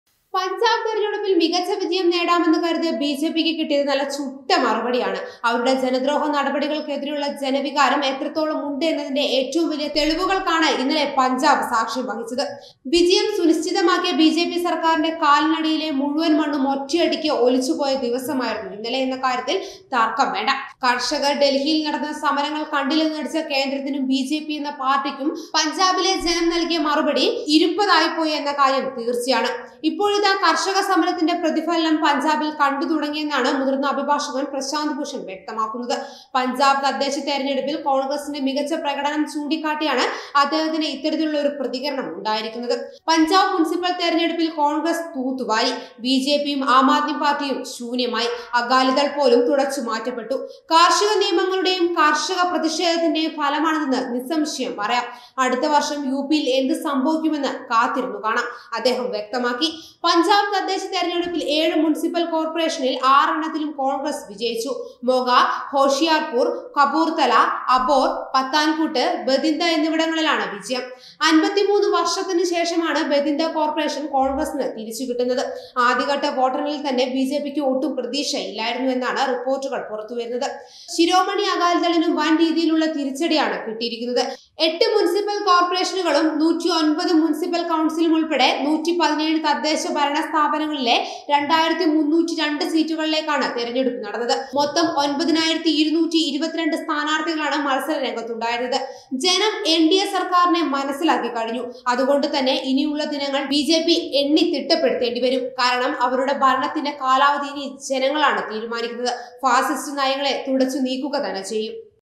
ഇത് കർഷകരുടെ വിജയം. ബിജെപിക്കാർക്ക് ഇനി കണ്ടംവഴി ഓടാം. ഇതിൽ കൂടുതൽ എന്ത് നാണക്കേട് ആണ് ഭായ്.
പഞ്ചാബിൽ തെരഞ്ഞെടുപ്പിൽ മികച്ച വിജയം നേടാമെന്ന് കരുതിയ ബിജെപിയ്ക്ക് കിട്ടിയത് നല്ല ചുട്ട മറുപടിയാണ്. അവരുടെ ജനദ്രോഹ നടപടികൾക്ക് എതിരെയുള്ള ജനവികാരം എത്രത്തോളം ഉണ്ട് എന്നതിന്റെ ഏറ്റവും വലിയ തെളിവുകൾക്ക് ആണ് ഇന്ന് പഞ്ചാബ് സാക്ഷ്യം വഹിച്ചത്. വിജയം സുനിശ്ചിതമാക്കിയ ബിജെപി സർക്കാരിന്റെ കാലിനടിയിലെ മുഴുവൻ മണ്ണും ഒറ്റയടിക്ക് ഒലിച്ചുപൊയ ദിവസമായിരുന്നു ഇന്നലെ എന്ന കാര്യത്തിൽ തർക്കം വേണ്ട. കർഷകർ ഡൽഹിയിൽ നടത്തുന്ന സമരങ്ങൾ കണ്ടില്ലെന്നു നടിച്ച കേന്ദ്രത്തിനും ബിജെപി എന്ന പാർട്ടിക്കും പഞ്ചാബിലെ ജനം നൽകിയ മറുപടി ബിജെപി എന്ന പാർട്ടിക്ക് ഇരുപ്പത് ആയിപോയി എന്ന കാര്യം തീർച്ചയാണ്.ഇപ്പോഴിതാ കര്‍ഷക സമരത്തിന്റെ പ്രതിഫലനം പഞ്ചാബില്‍ കണ്ടുതുടങ്ങിയെന്നാണ്
മുതിര്‍ന്ന അഭിഭാഷകന്‍ പ്രശാന്ത് ഭൂഷണ്‍ വ്യക്തമാക്കുന്നത്.. പഞ്ചാബ് തദ്ദേശ തെരഞ്ഞെടുപ്പില്‍ കോണ്‍ഗ്രസിന്റെ മികച്ച പ്രകടനം ചൂണ്ടിക്കാട്ടിയാണ് അദ്ദേഹത്തിന്റെ ഇത്തരത്തിൽ ഉള്ള ഒരു പ്രതികരണം ഉണ്ടായിരിക്കുന്നത്.”പഞ്ചാബ് മുനിസിപ്പല്‍ തെരഞ്ഞെടുപ്പില്‍ കോണ്‍ഗ്രസ് തൂത്തുവാരി.